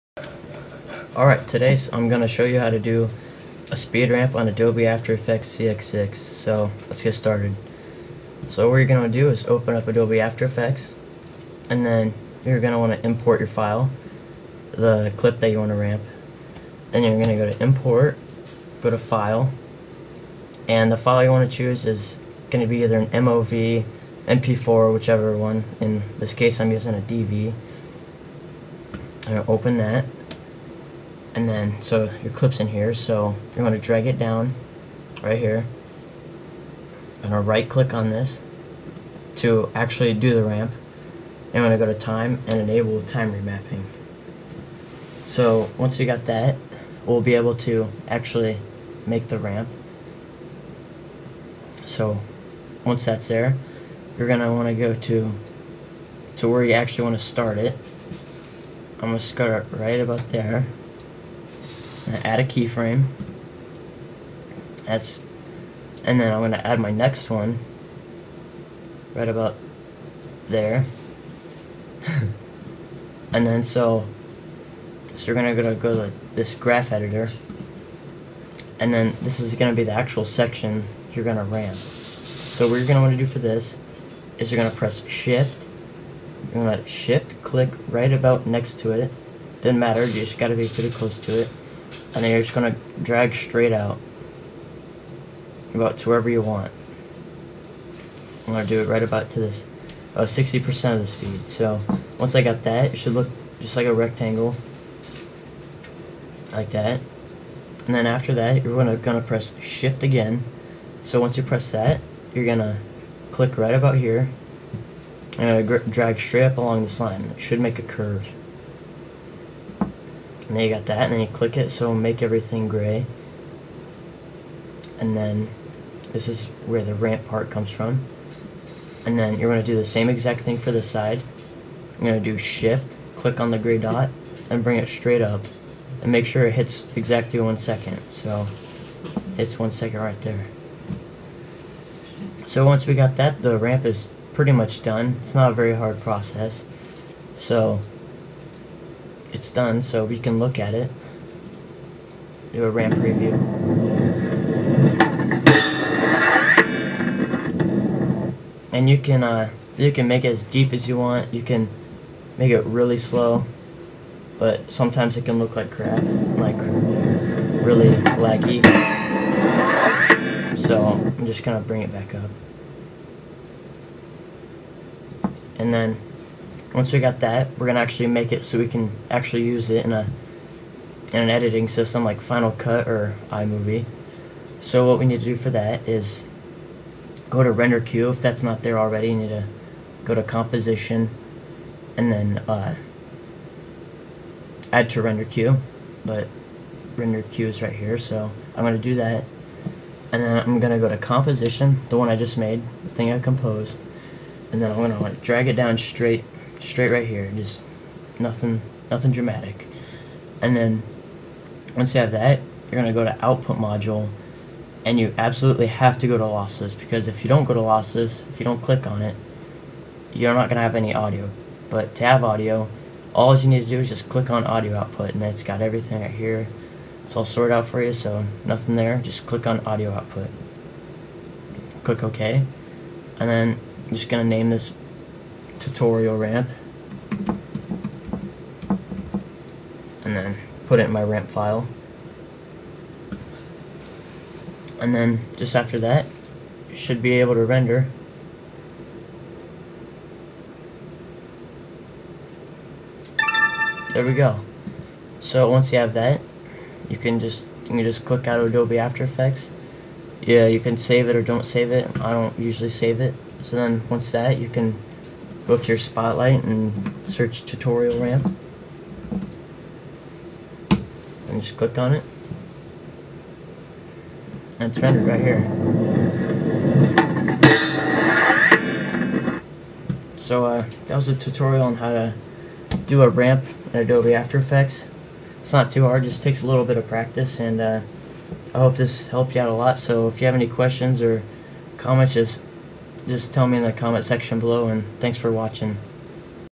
How to Ramp with Adobe After Effects CS6 (Tutorial)
Just a tutorial on ramping clips. If you have any questions just leave a comment or message me. HOPE IT HELPS!!!